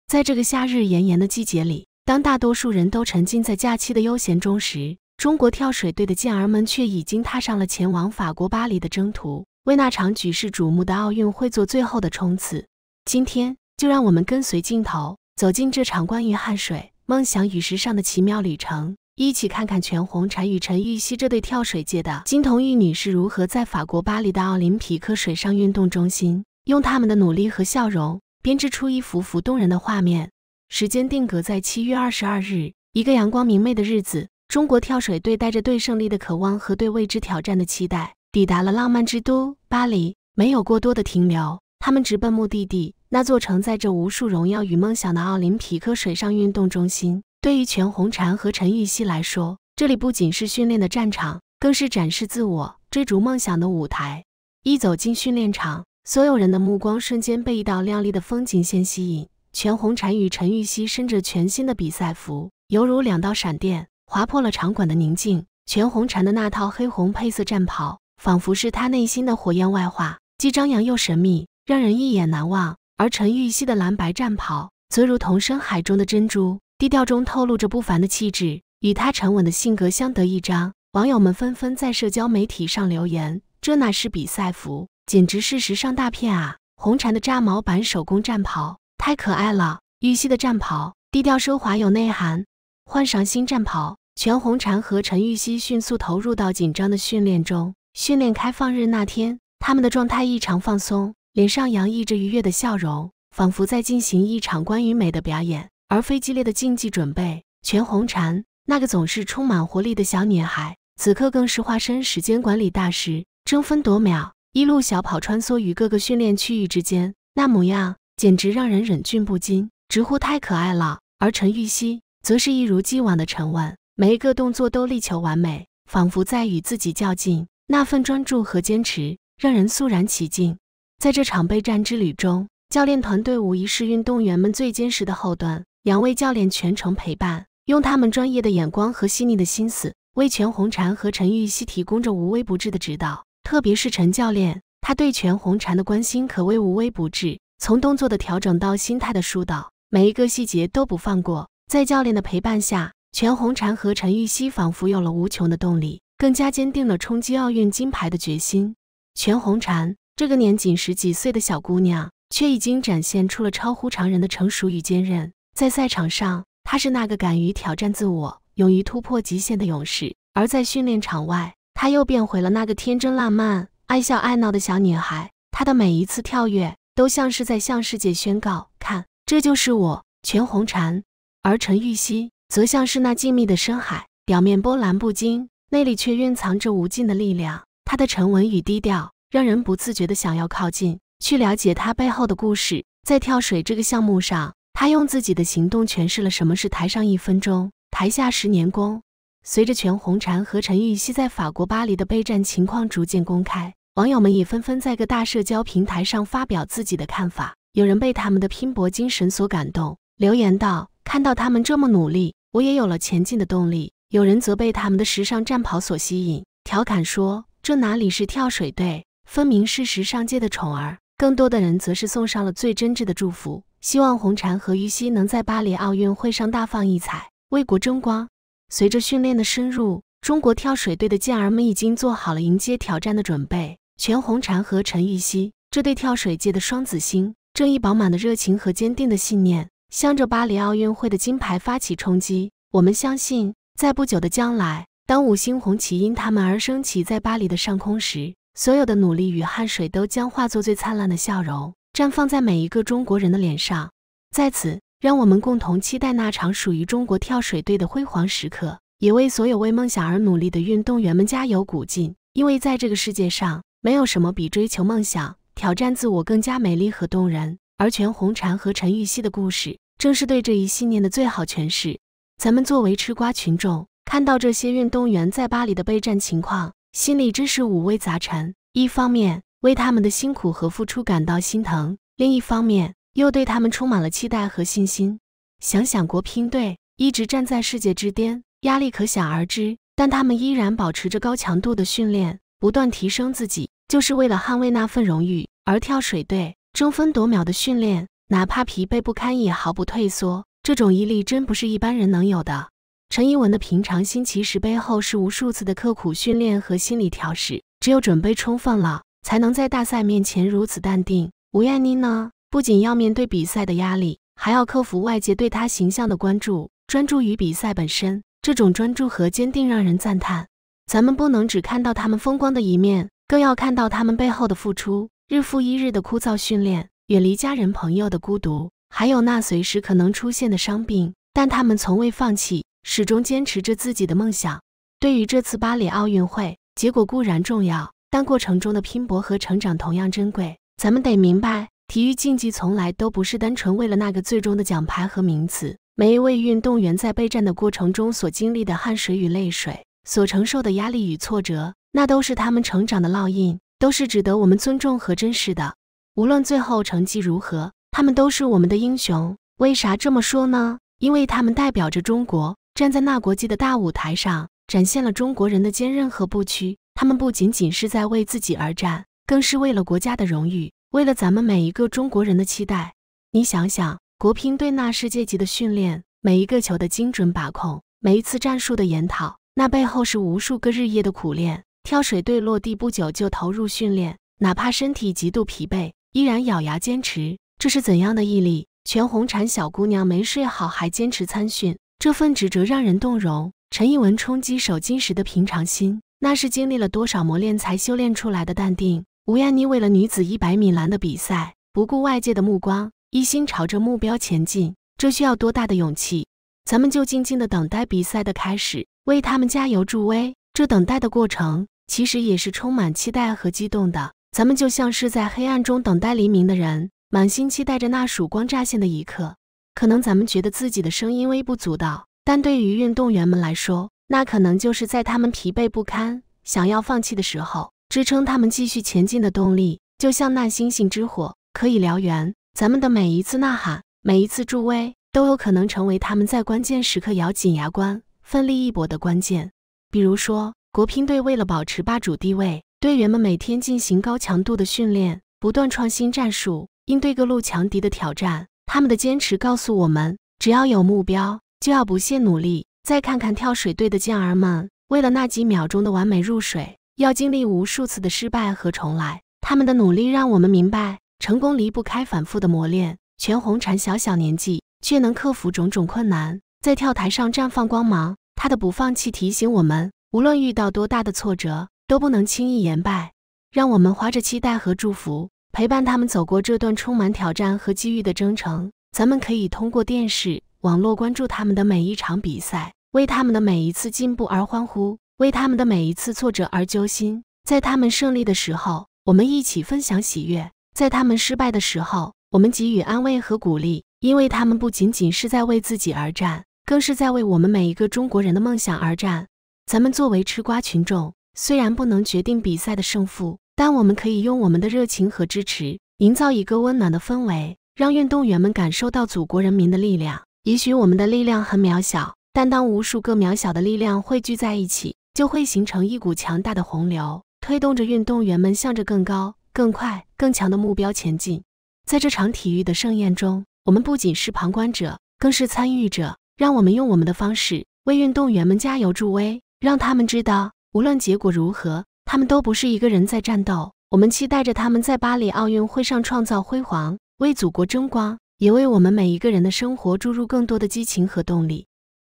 巴黎奧運會，中國跳水隊最新比賽服也太帥了！全紅蟬爭分奪秒抓緊時間去訓練！全紅婵
巴黎奧運會，中國跳水隊最新比賽服也太帥了！全紅蟬爭分奪秒抓緊時間去訓練！